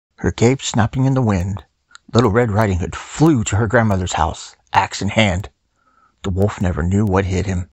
One Minute Fairy Tales – Red Riding Hood
My wife and I have decided to not have children, so, bibliophiles that we are, I decided to read bedtime stories to other people's children.

Join me this time for _Uncle FAuX's One-Minute Fairy Tale: Red Riding Hood_.